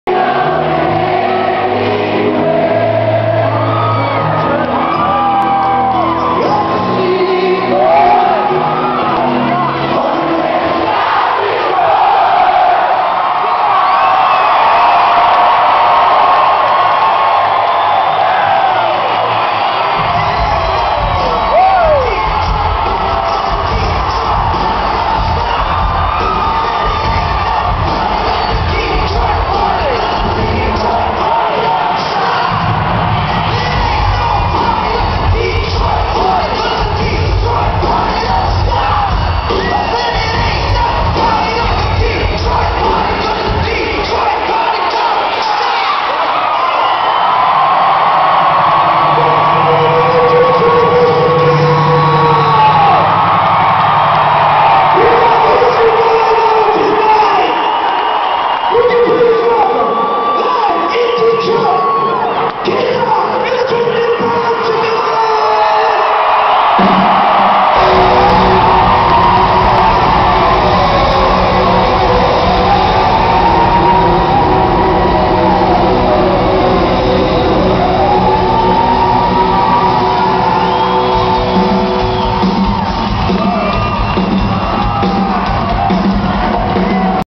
Kid Rock @ Comerica Park 2009 (Intro)
Kid Rock. Opening night at Comerica Park,  Detroit
Friday July 17th 2009